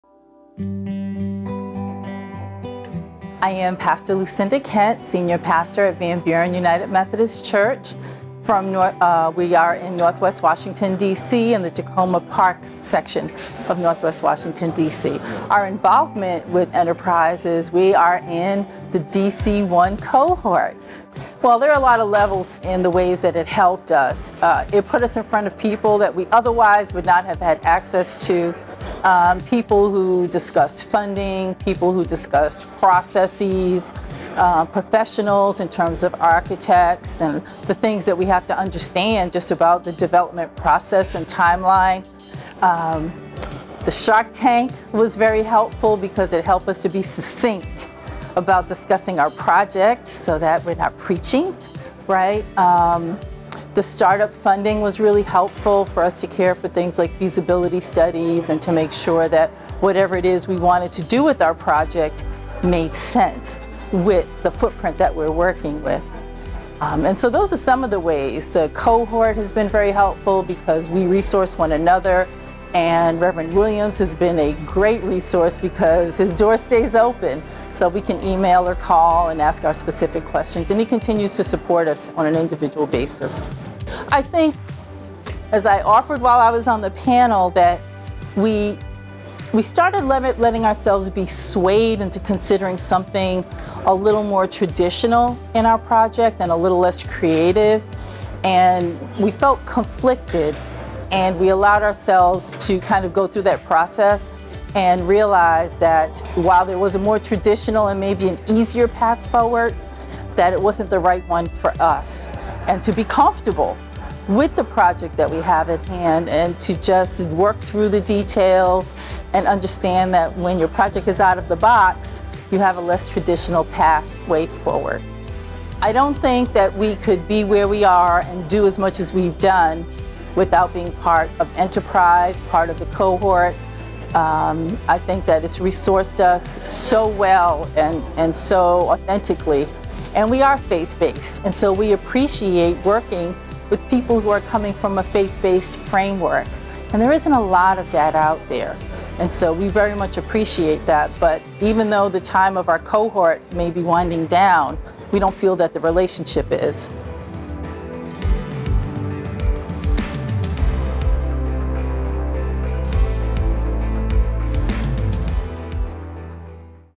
FBDI Takeaways: Pastor Lucinda Kent
Pastor Lucinda Kent of Van Buren United Methodist Church shares what she gained as a participant of Enterprise’s Faith-Based Development Initiative DC cohort.